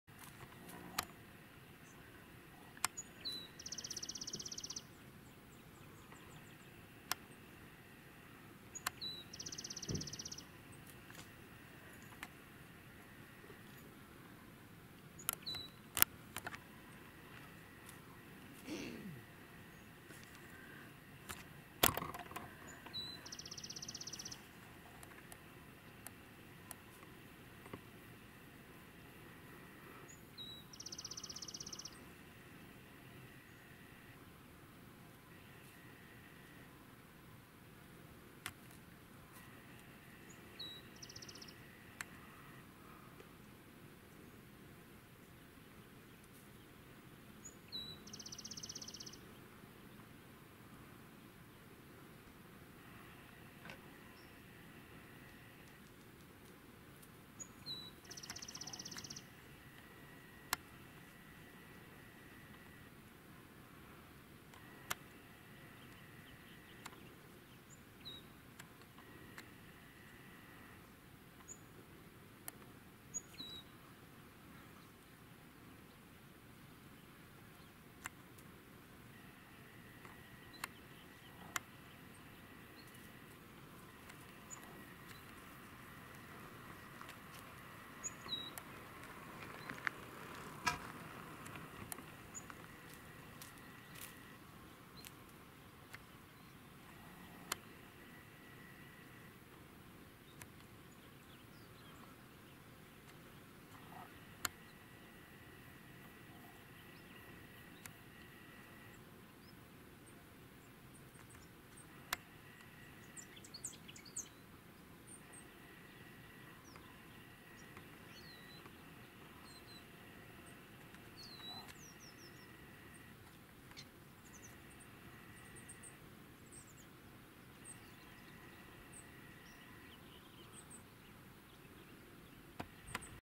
Lizard Eats Sphinx Moth Caterpillar
I got to watch this Lizard eat a Sphinx Moth Caterpillar at Catalina State Park on August 28, 2019.